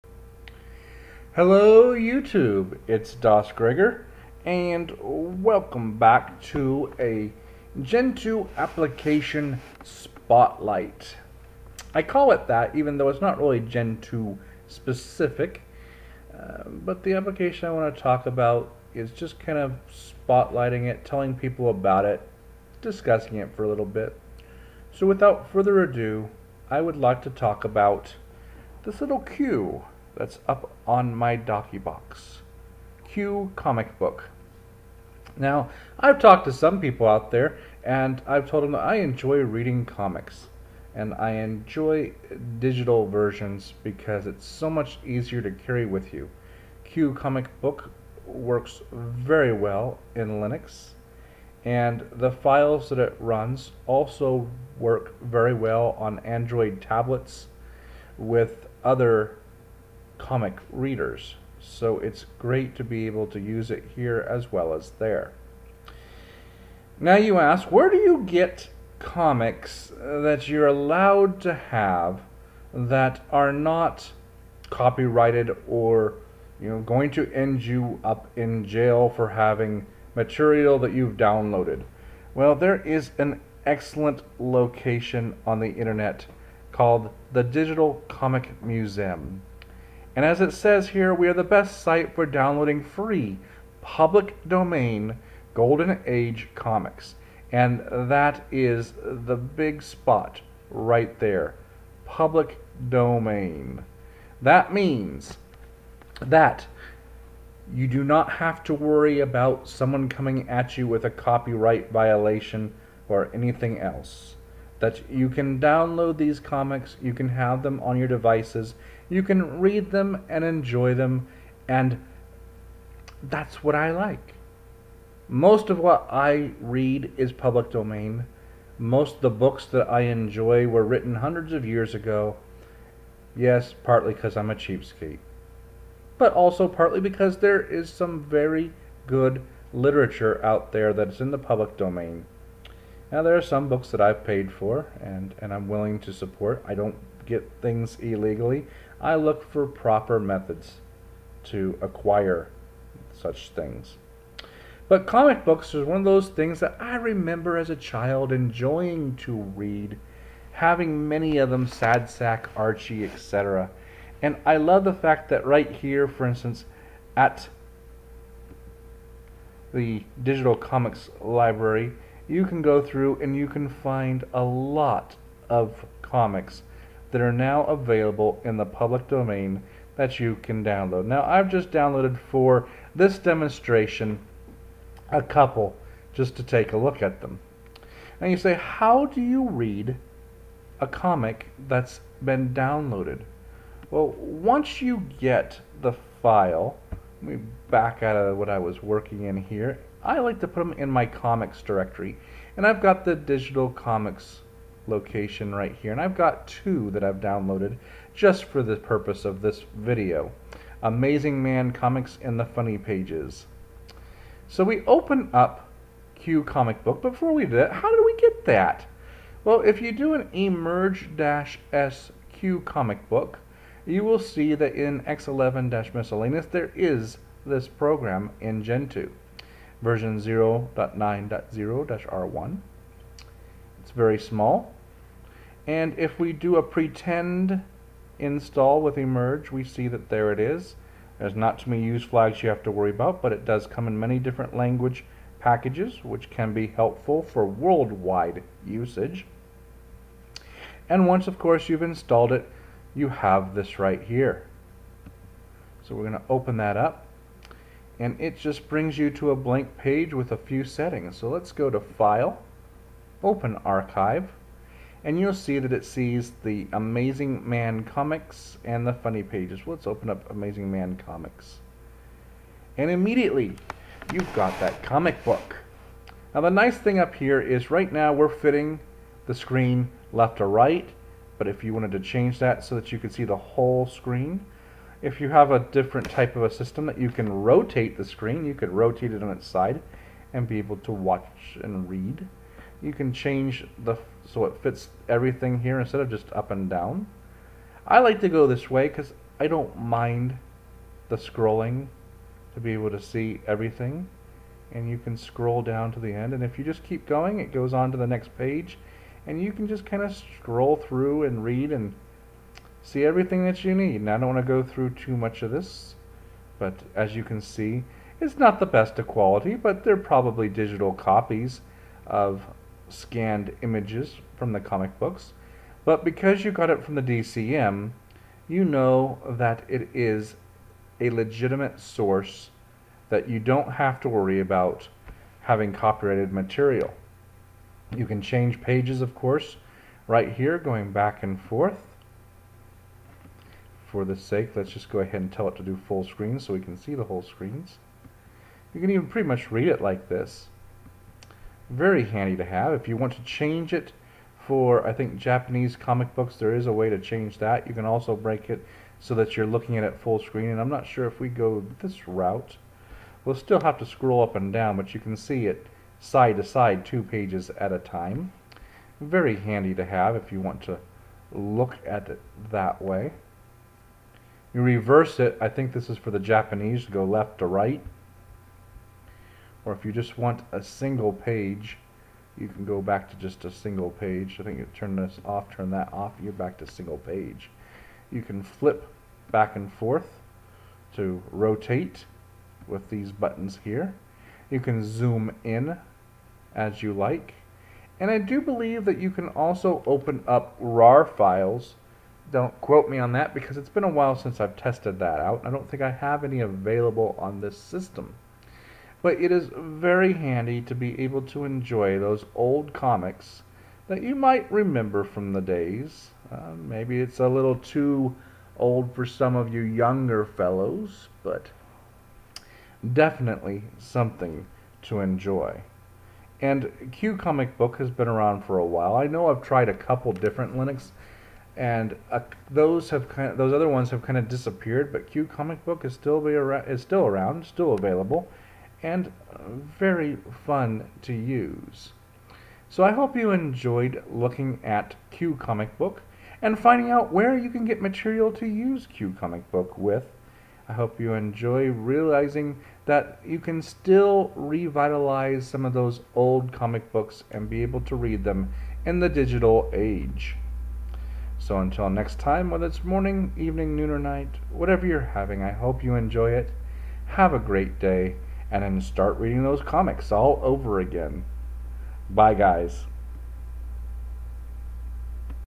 Gentoo Tidbits - QComicBook
I recorded this a few weeks ago, but wanted to share with you.

Finally an awesome comic book reader and viewer in Linux!!  Read lot's of your old favouites with this app.  Find your favourite public domain comics at The Digital Comic Museum and start enjoying them now.